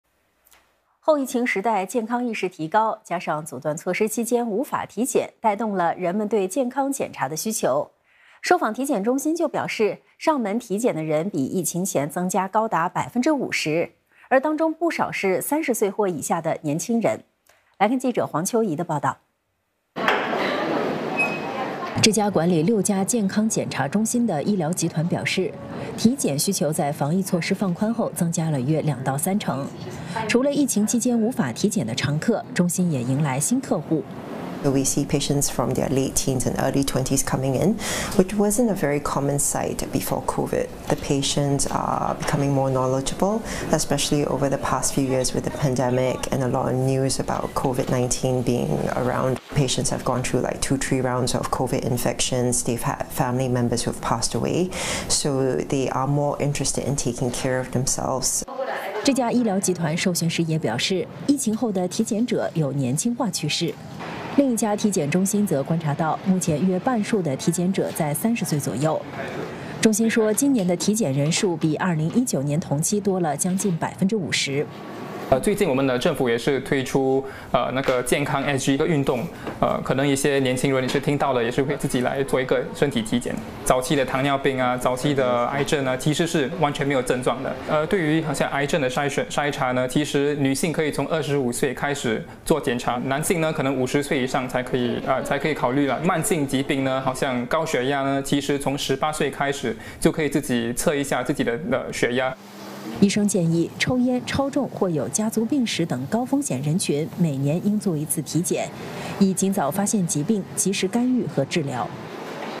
后疫情时代体检需求增加 有年轻化趋势
后疫情时代健康意识提高，加上阻断措施期间无法体检，带动了人们对健康检查的需求。